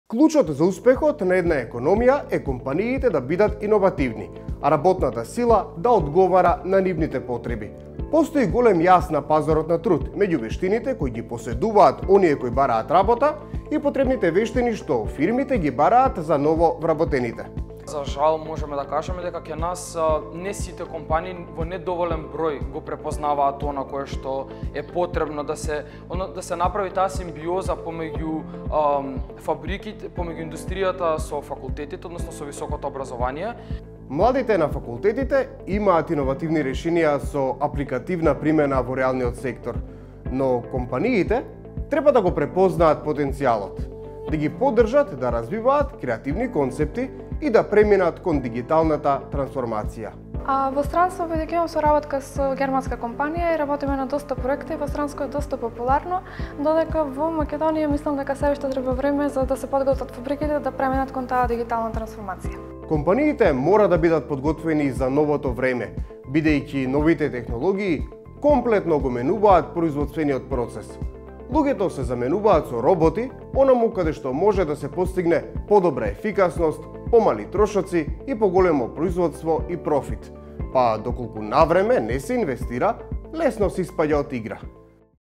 Компаниите не го препознаат потенцијалот на младите иноватори
Постои голем јаз на пазарот на труд меѓу вештините на оние кои бараат работа и вештините што им се потребни на фирмите.